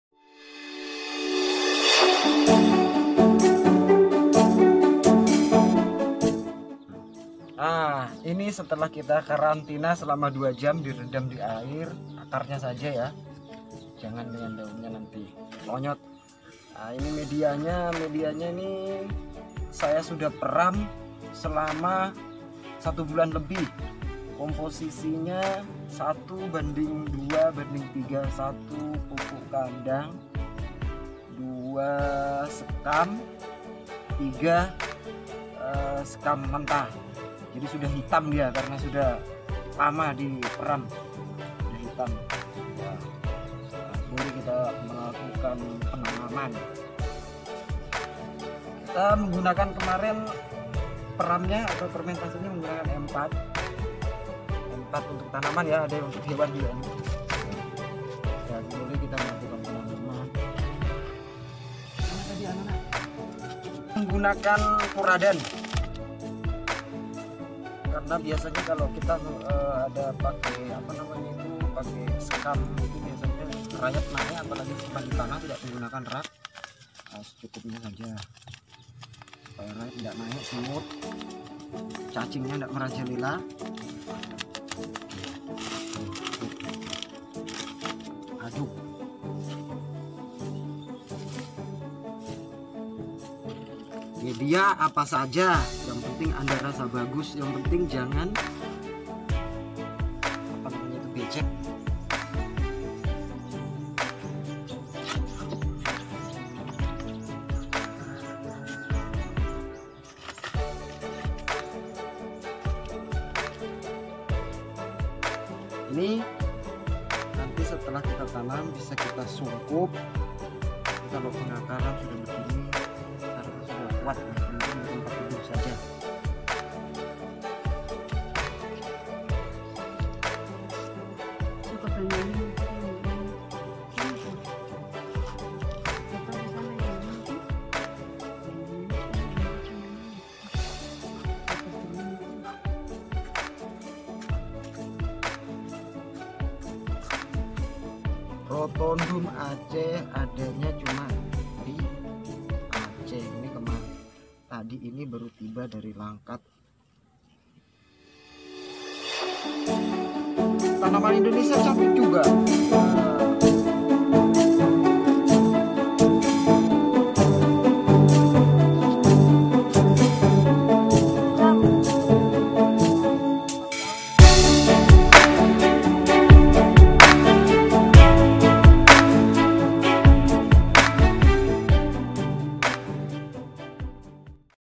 Media Tanam ROTUNDUM ACEH agar cepat besar Dan Subur
Media Tanam ROTUNDUM ACEH
agar cepat besar dan Subur.kita menggunakan MEDIA TANAM  yang sudah difermentasi..